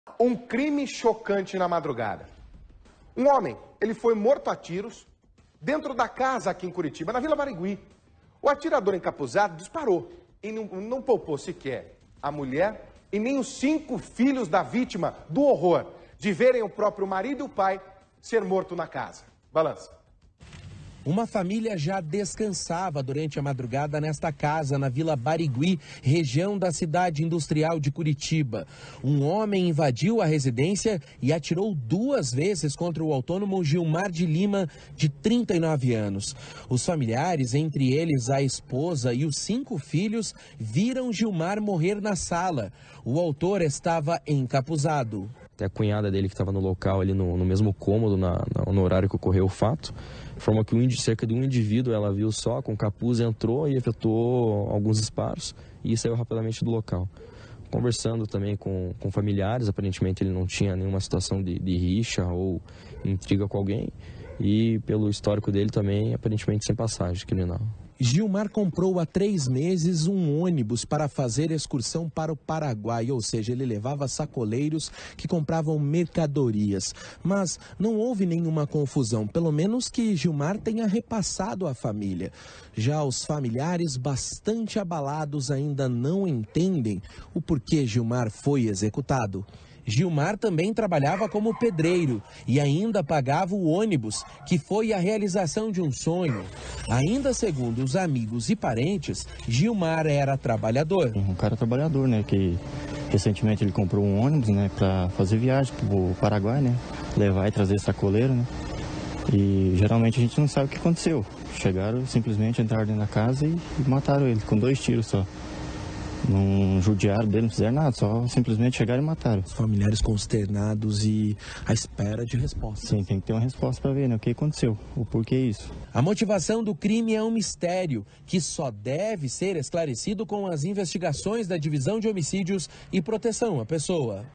Homem é morto por atirador encapuzado dentro de casa na frente da família no CIC
Durante a madrugada, na Vila Barigui, no CIC, um homem encapuzado invadiu uma casa e atirou duas vezes contra o morador. A esposa e os cinco filhos da vítima viram o assassinato.